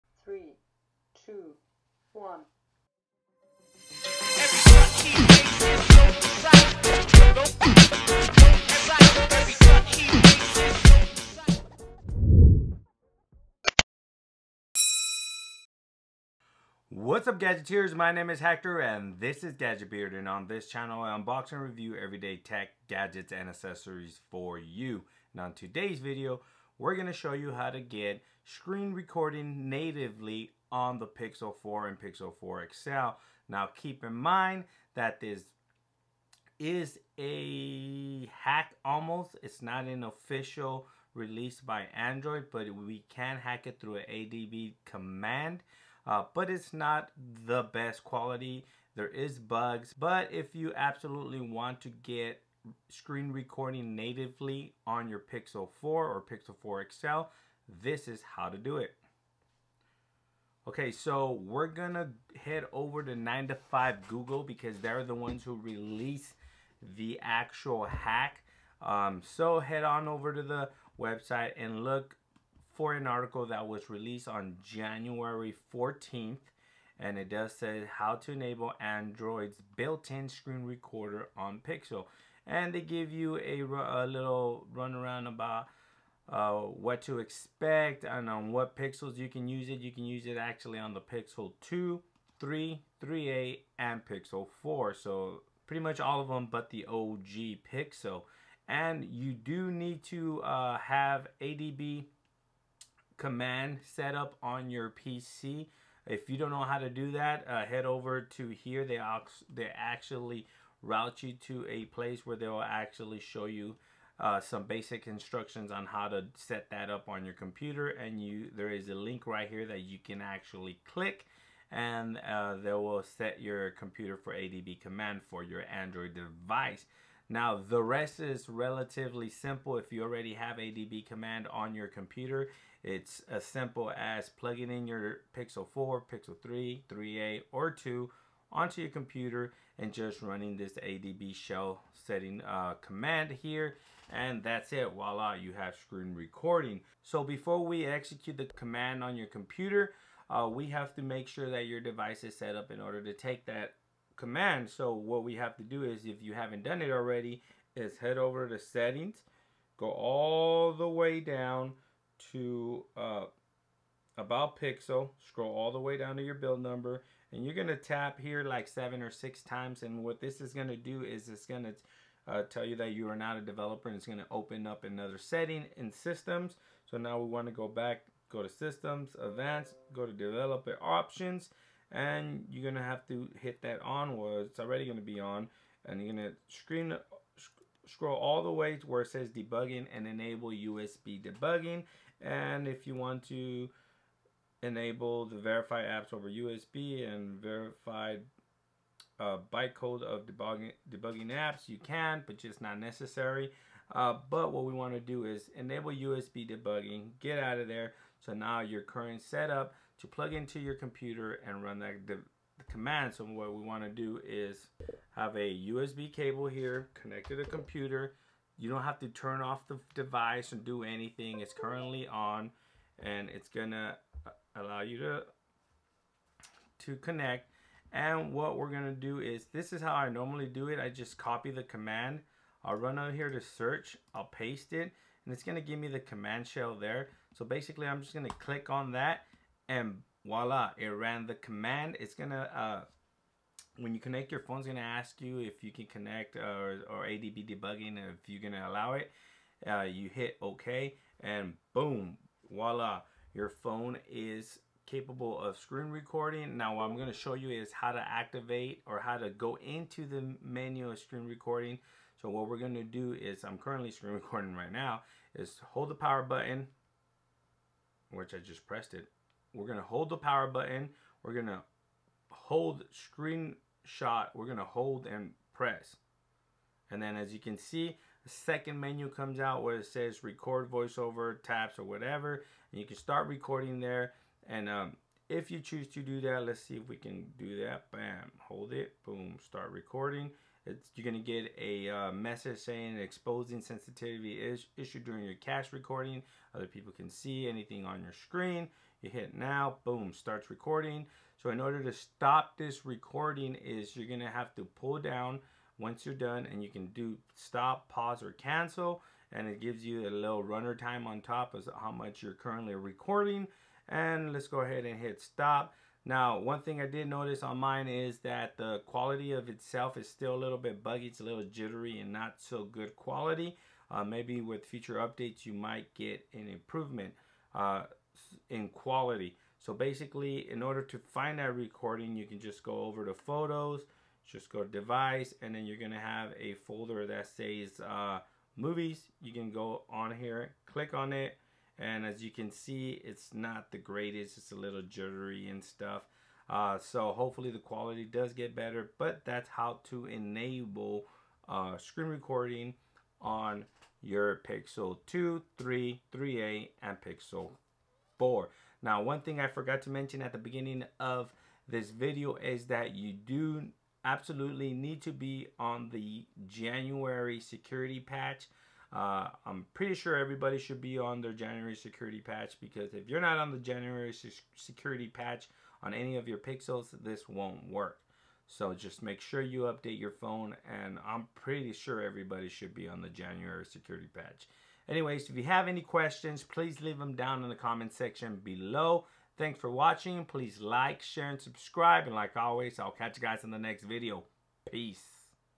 Native Screen Recording On Pixel 4
This is a quick method to enable native screen recording for the Google Pixels Smartphones...

Google Pixel 4 XL (Amazon)

Amazon Store:
Try Twitch Prime:

Amazon Wish List :